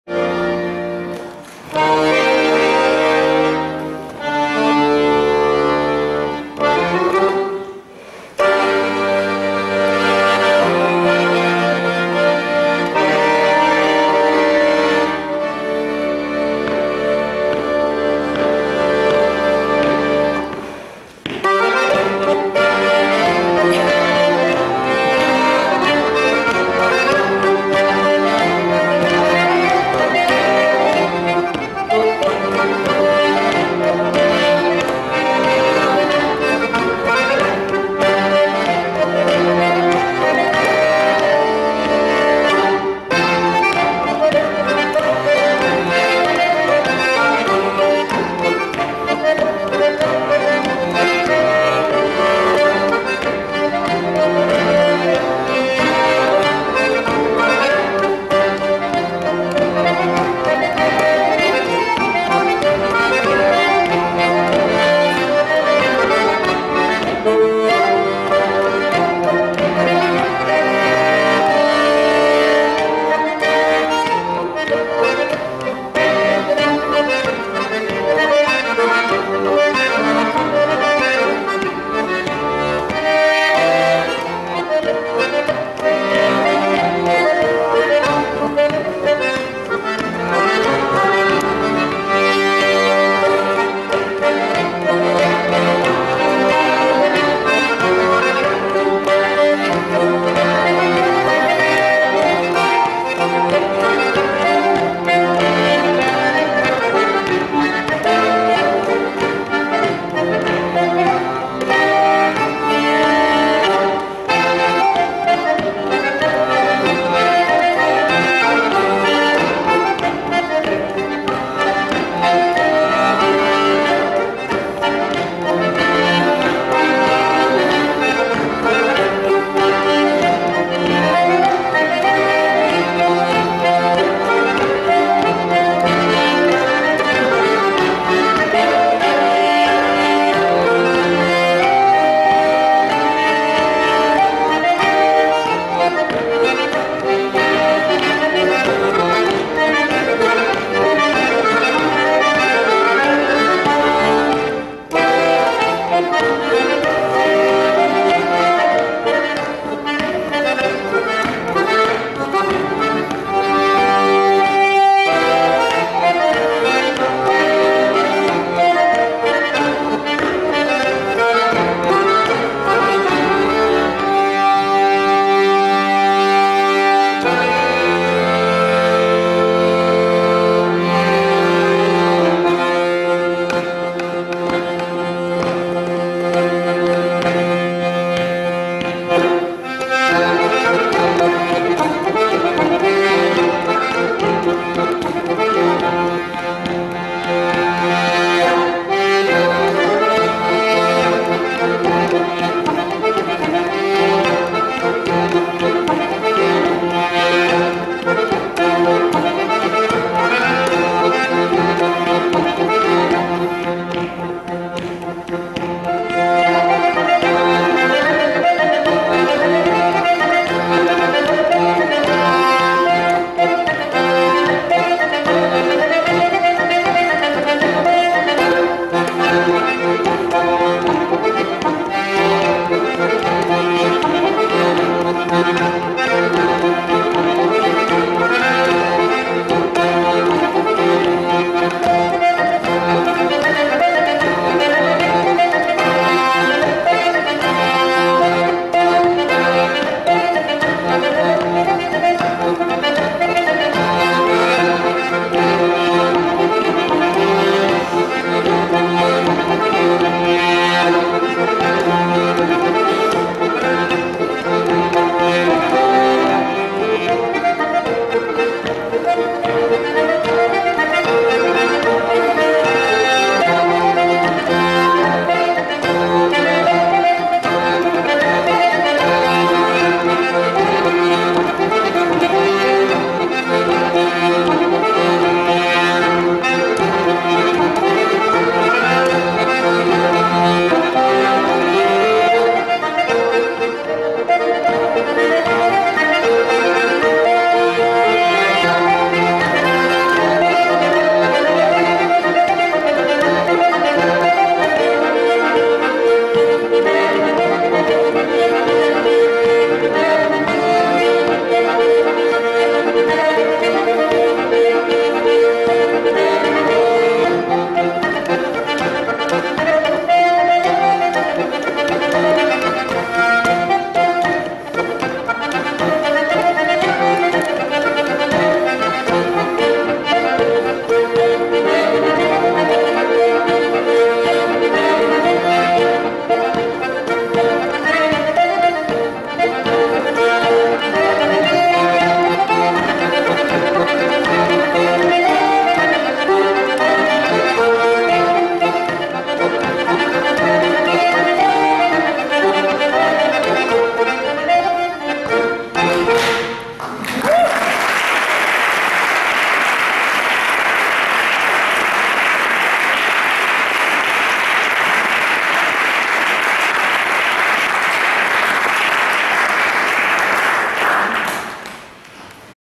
Nuit du Folk Dioise 2018 - Christian Pacher - Concert au Temple (1)
Au cours de la Nuit du Folk Dioise 2018, située à Châtillon en Diois, Christian Pacher a donné un magnifique concert au temple.